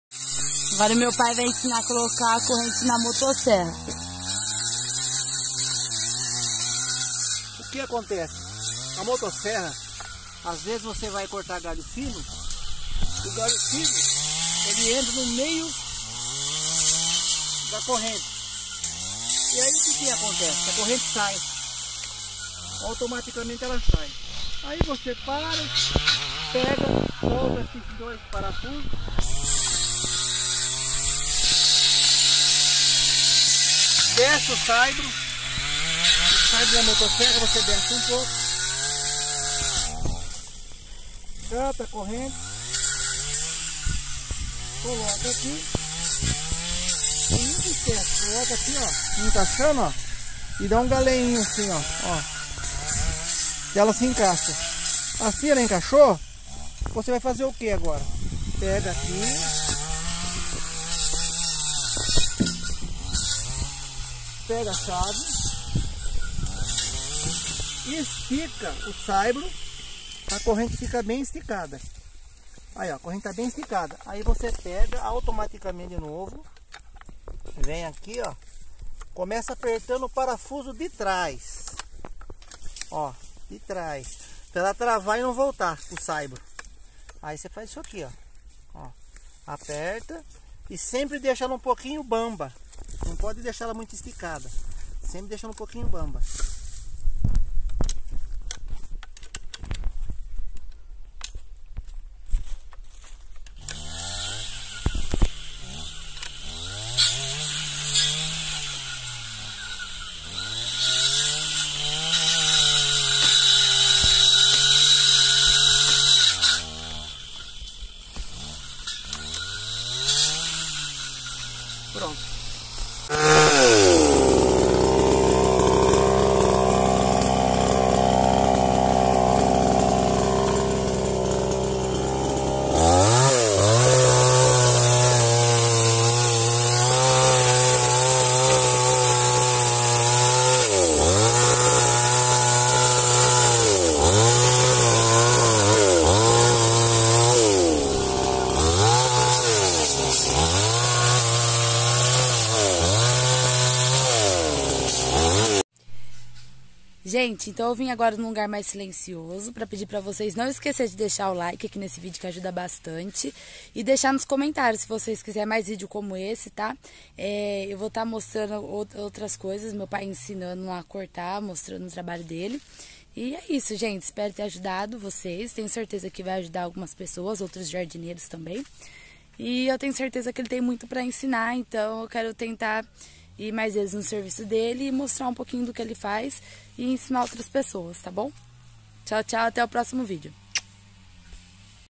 COMO COLOCAR CORRENTE NA MOTOSSERRA (Carlos Jardineiro)
Neste vídeo meu pai está ensinando a colocar a corrente na motocerra quando ela solta na hora que você está usando no trabalho, ele também explica o porque que acontece dela soltar.